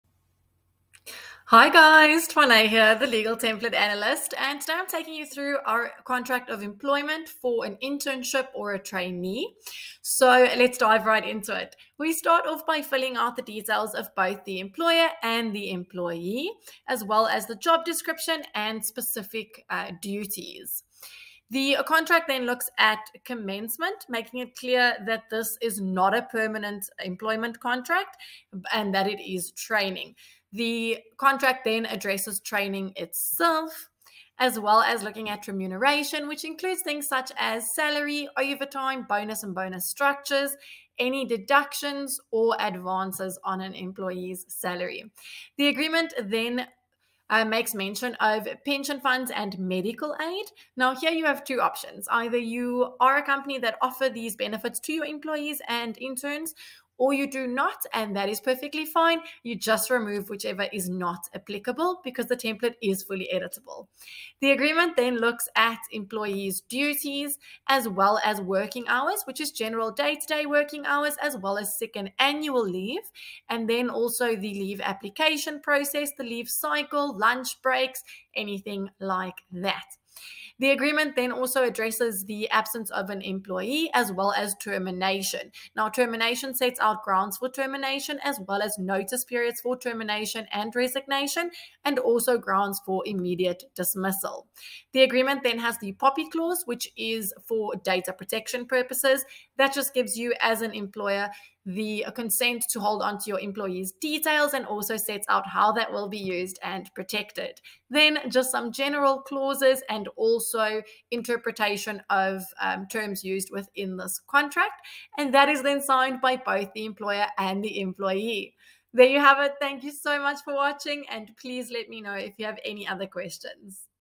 Employment Contract Internship or Trainee
Come and take a peak at our Employment Contract for an Internship or Trainee with me!

Intern or trainee employment agreement detailing duties, duration, compensation, confidentiality, and termination provisions.